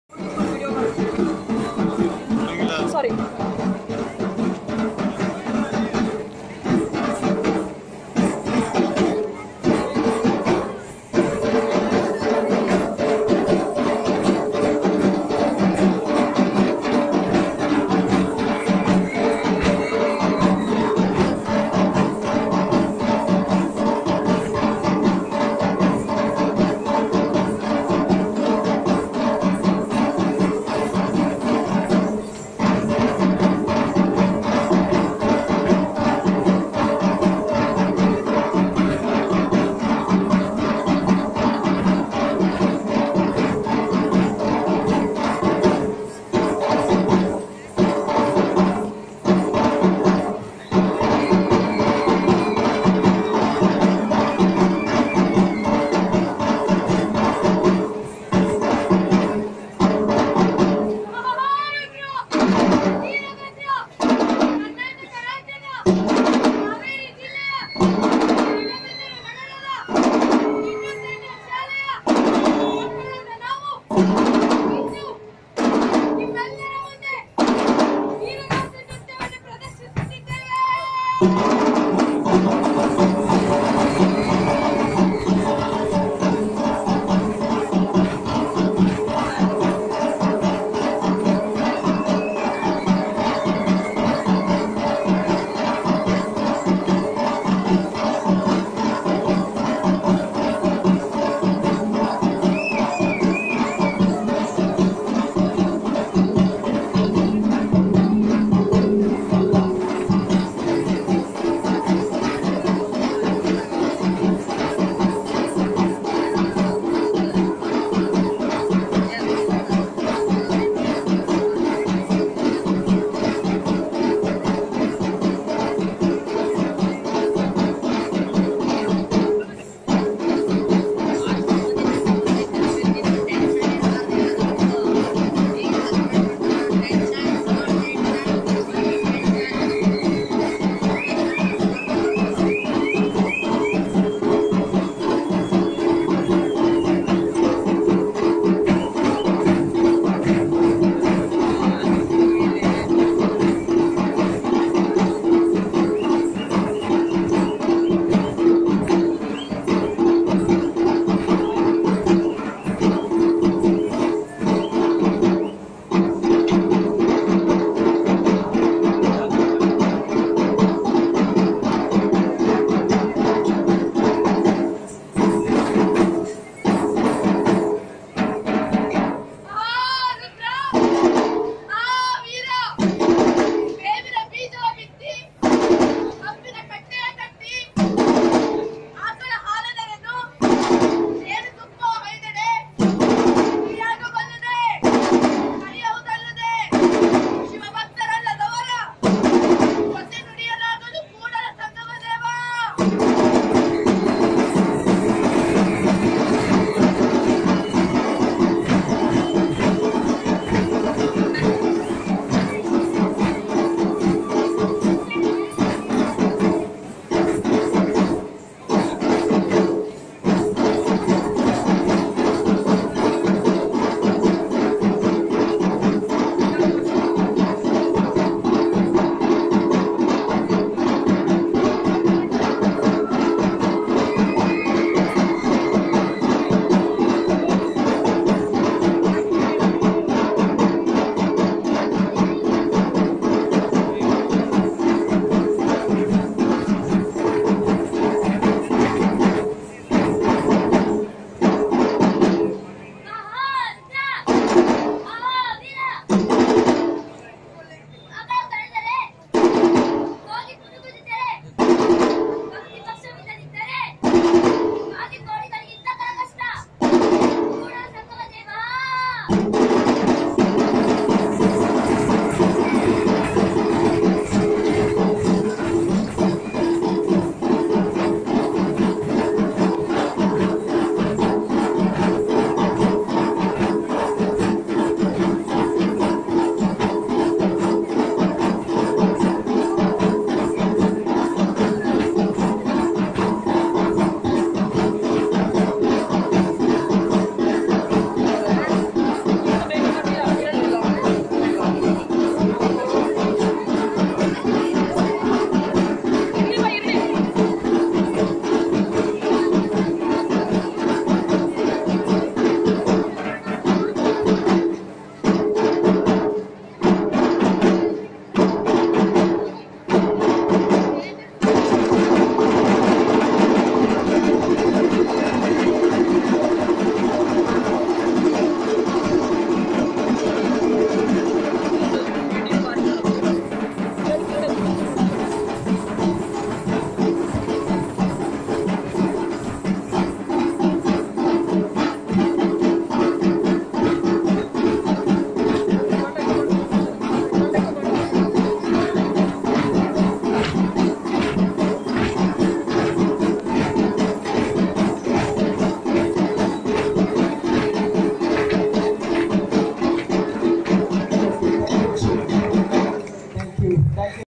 Veeragase an energetic dance form, in which we won 1st place. feeling proud.
This dance form got 1st place at PSSEMR Davanagere.